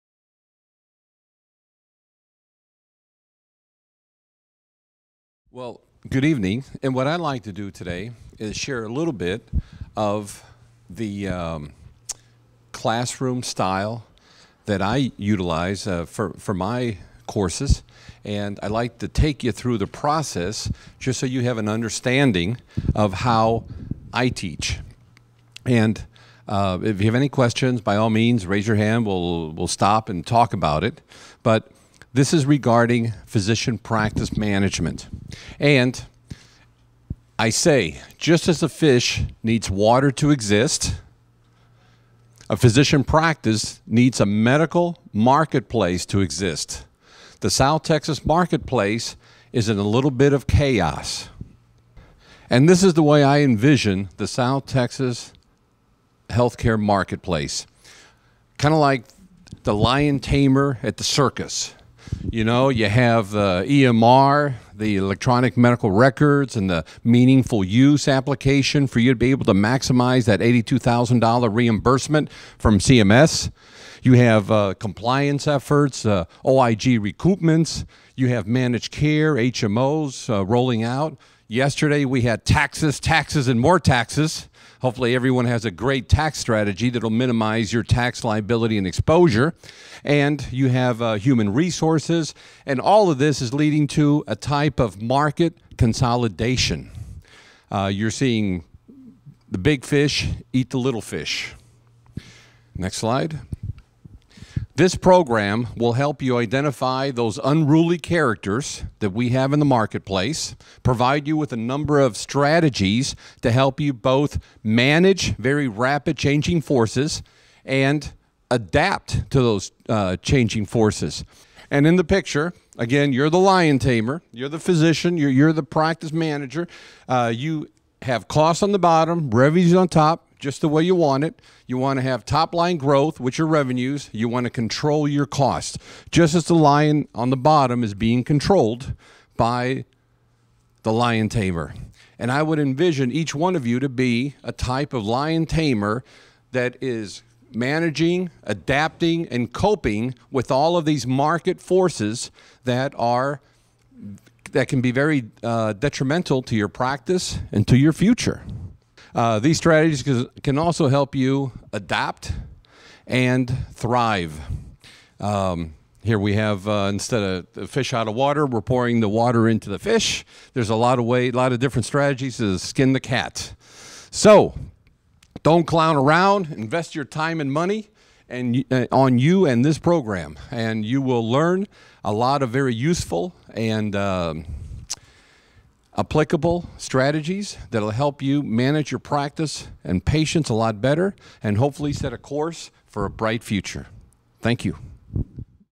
Health Practice Management Module 1
The University of Texas-Pan American's Office of Continuing Education presents the Executive Certificate Program in Healthcare Practice Management.  Module 1 of the program focuses on Practice Management with Mario Garza, MSW, MBA.

This Executive Practice Management Certificate Program is designed for healthcare practice managers including but not limited to Physicians, PAs (Physician Assistants), FNPs (Nurse Practitioners), Pharmacists, and Mental Health Practioners. This program is offered by UTPA Continuing Education at the UTPA McAllen Teaching Site.